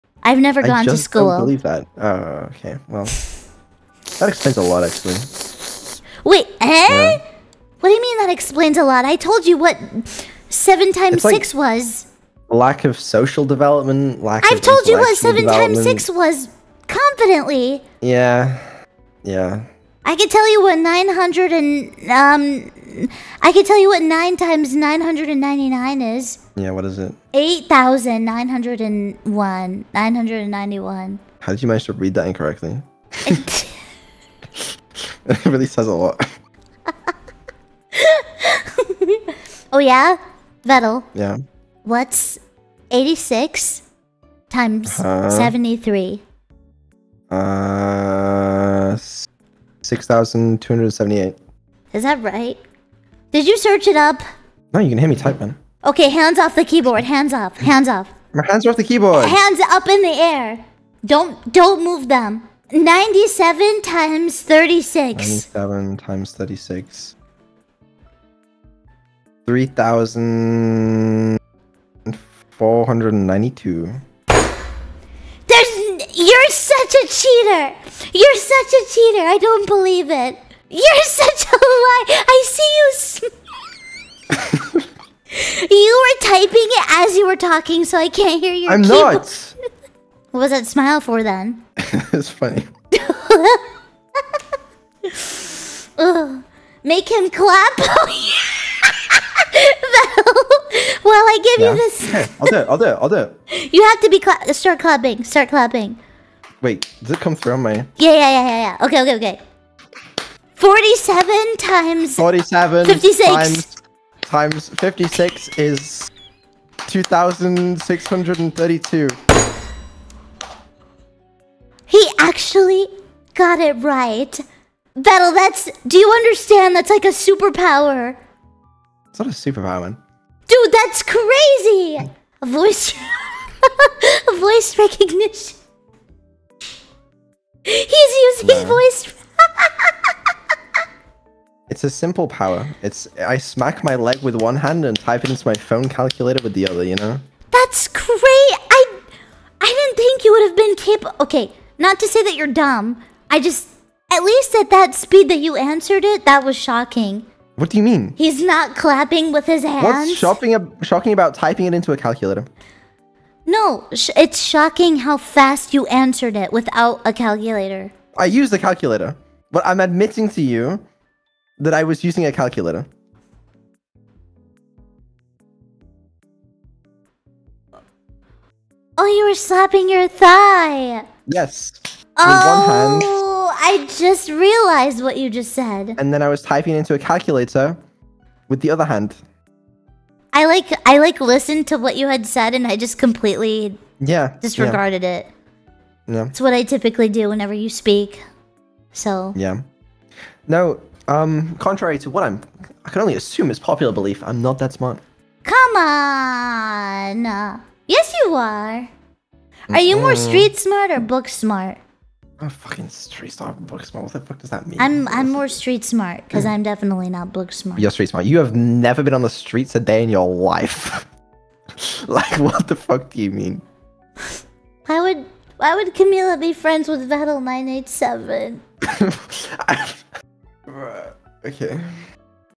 Camila tested Vedal's math skills without using a calc
btw calc is short for calculator